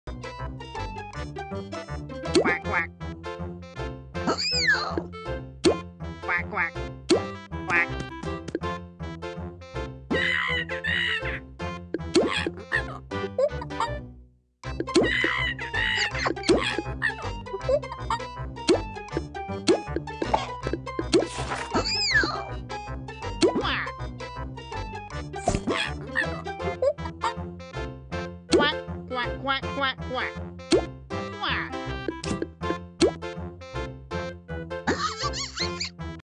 Pearl Angel Uber Gets Lawbot Suit
Pearl Angel Uber finally getting Lawbot suit!